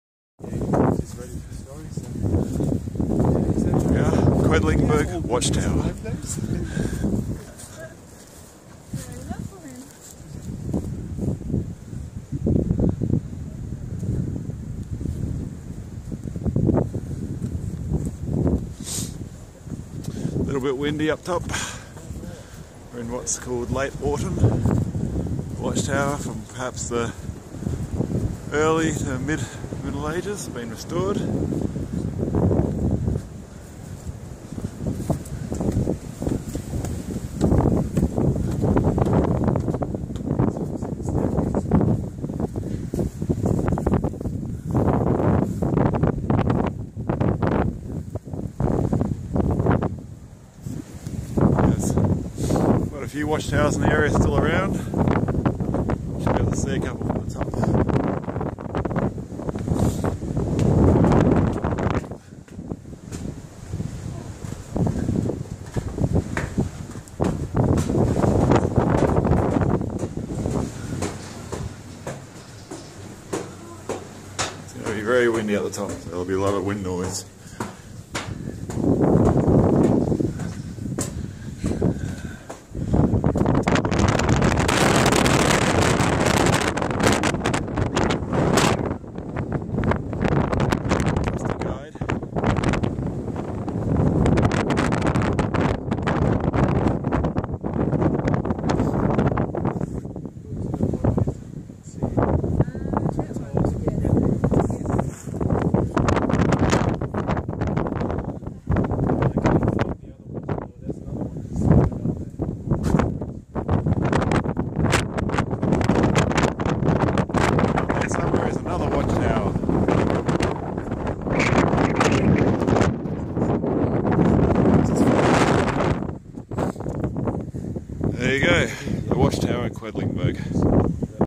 Quedlinburg watch tower
Climbing the old but recently renovated watch tower near Quedlinburg.